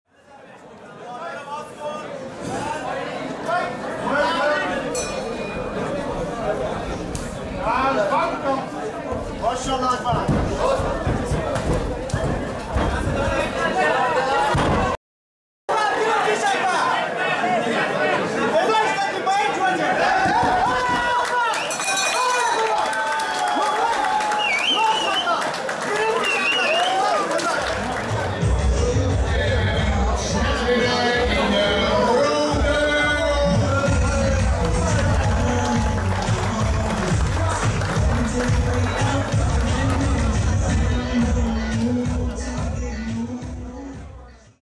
FIGHTCLUB DEN HAAG ON TOUR - Ali Akbar Momadi vs Adam Kandoussi
MMA amateur partij 3x3 minuten